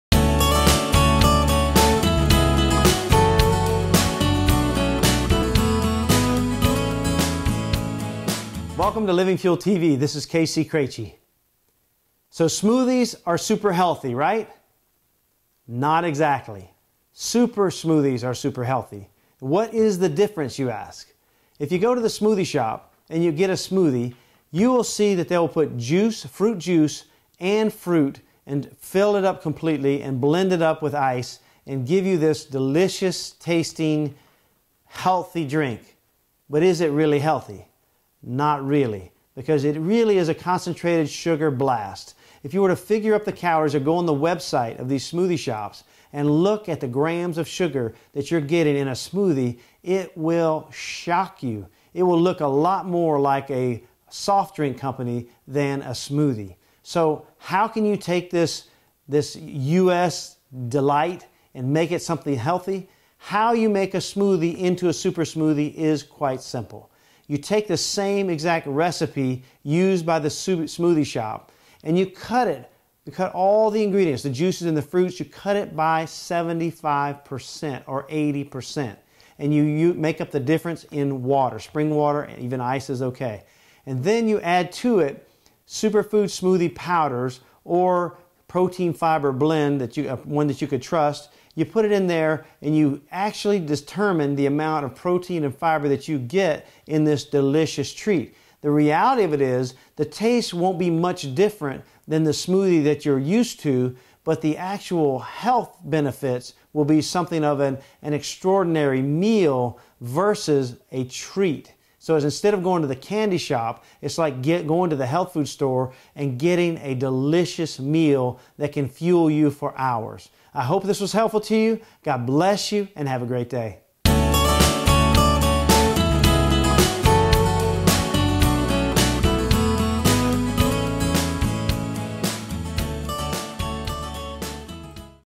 Sugar Smoothies to Super Smoothies
Orange Juice, Mango, Bananas, Electrolyte Mix, Turbinado, Honey

These are the ingredients in a "Stay Healthy" smoothie from a nationwide smoothie outlet that you would recognize. While their brightly-lit menu board proudly displays these wholesome, all-natural, healthy ingredients, you must dig further to uncover the astonishing nutritional facts. This "small" size (20 ounce) smoothie packs 465 calories, a stunning 108 grams of sugar, and just 1 gram of protein. This is over 25 teaspoons of sugar, or the sugar in 2 ½ conventional soft drinks. And these nutritional facts are the rule, not the exception for other smoothies marketed to "get energy", "shape up", or "trim down".

KC Craichy talks about how to modify conventional smoothie recipes, transforming them from sugar-laden treats into nutrient-rich super meals.